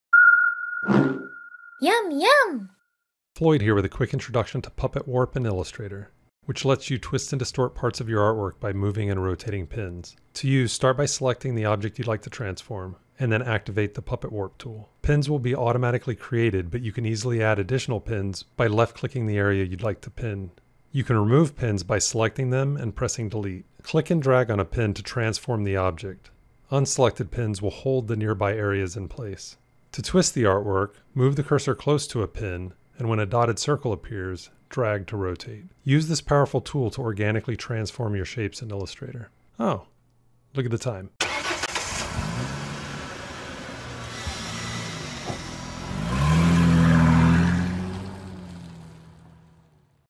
Illustrator | Puppet Warp
This video is a quick introduction to Puppet Warp in Illustrator, which lets you twist and distort parts of your artwork by moving and rotating pins.